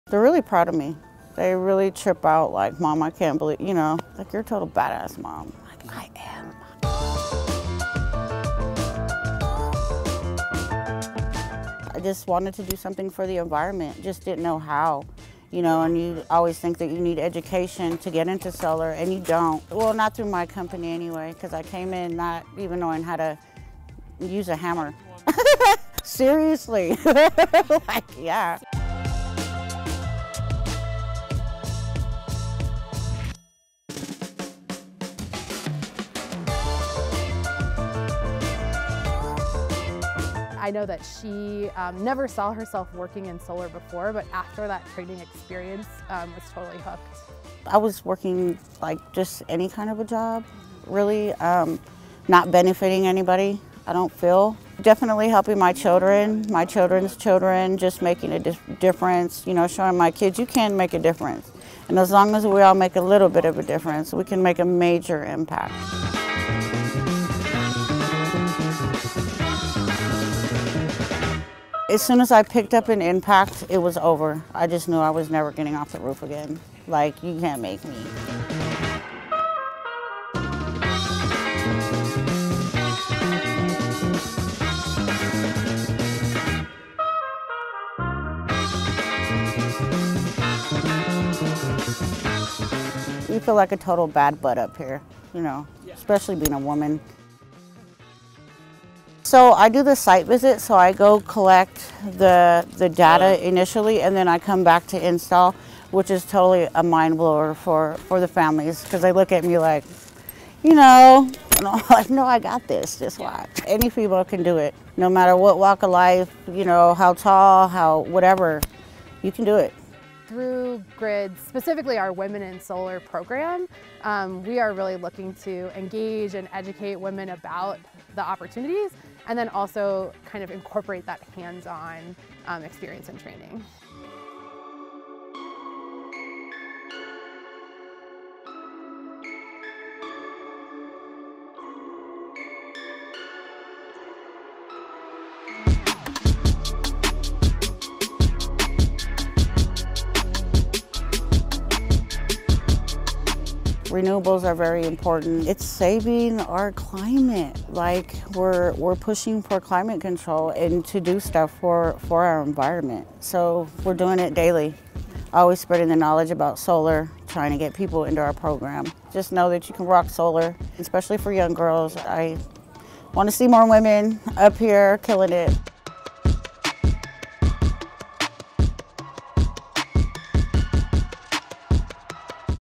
Solar Energy Is Creating New Jobs for a Sustainable Economy | NowThis
Here’s how the solar industry is creating countless new jobs while leading the way to a sustainable energy grid.

In US news and current events today, as we fight the climate crisis and climate change, moving away from fossil fuels will be a necessity. Enter solar power. This form of renewable energy is already creating tons of new jobs.